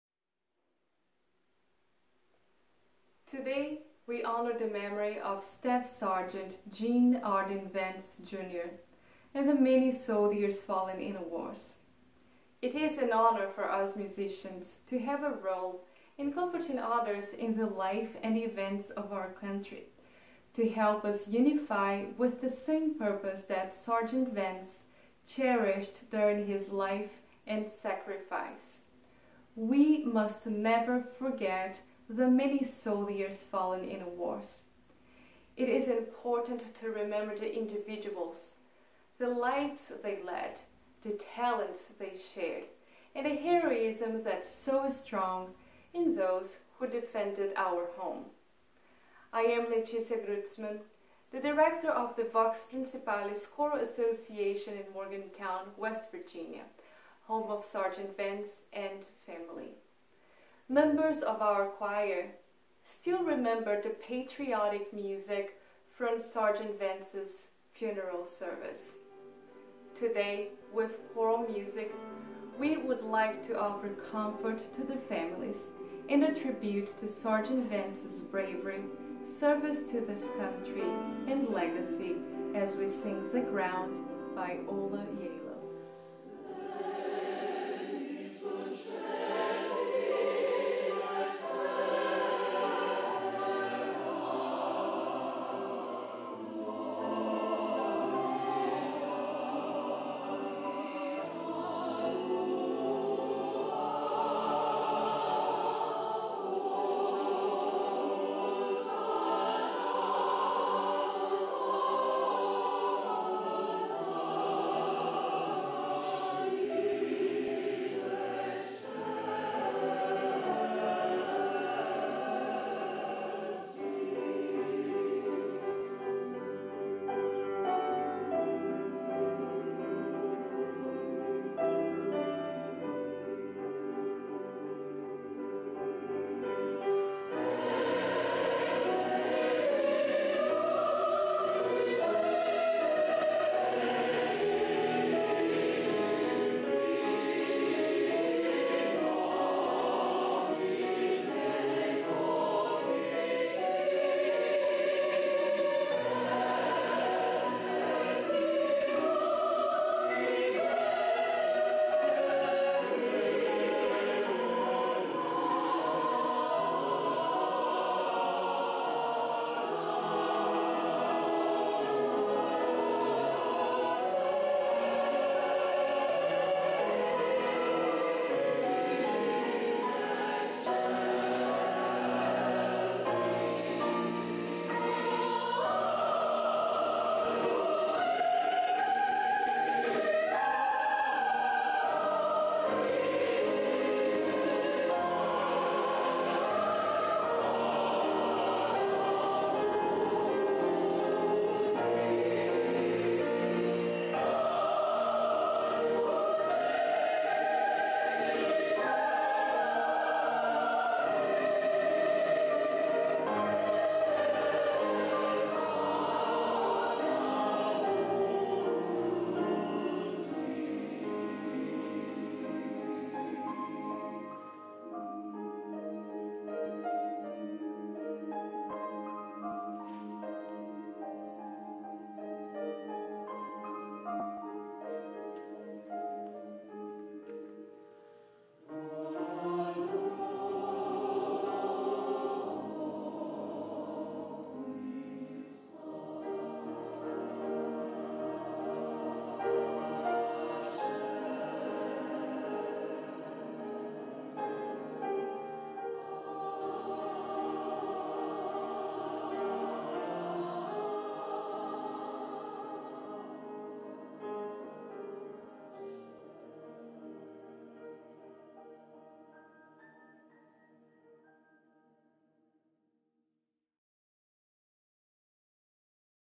Vox Principalis - Gene Arden Vance Jr. Memorial Tribute
Vox Principalis Choral Association
Letícia Grützmann, conductor
Yiwen Zhang, pianist
Video Editor, André Januário
Recorded in July 2019 at Spruce Street United Methodist Church